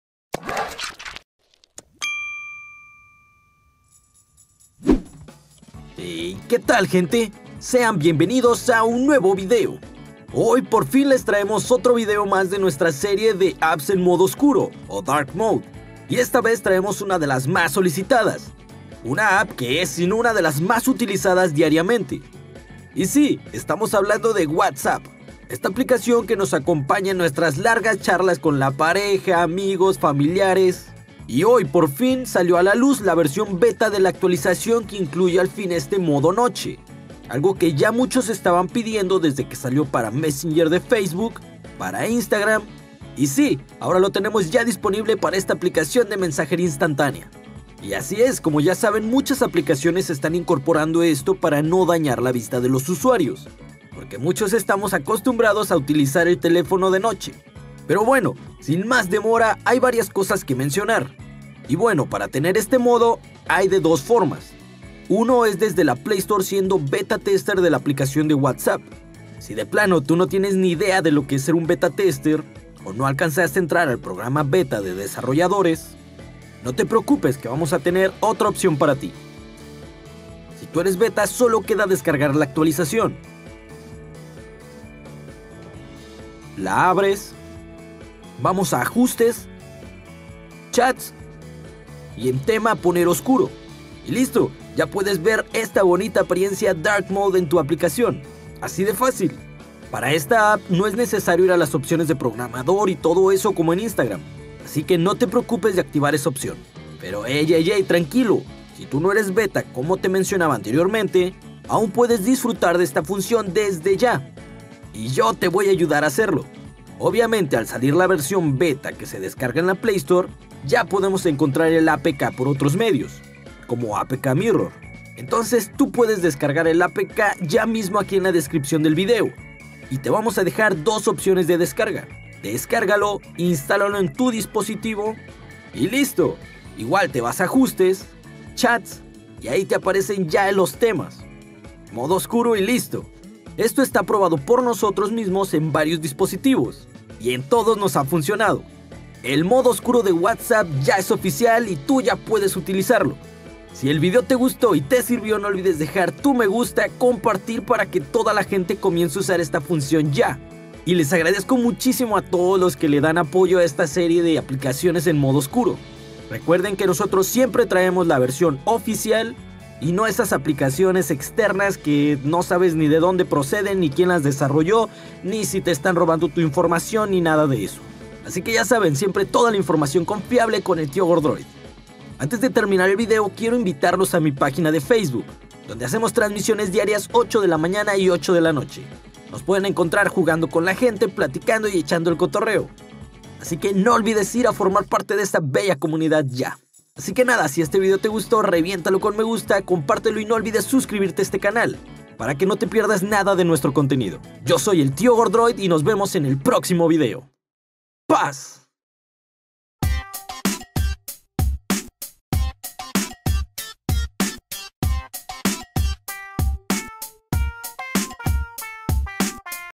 Activar WHATSAPP DARK MODE - MODO OSCURO Ya Disponible OFICIAL!
Hey que tal gente!
Sean bienvenidos a un nuevo vídeo!
Esta vez les mostramos como hacer WhatsApp MODO OSCURO!
Espero el vídeo les guste y les funcione!

PAZ!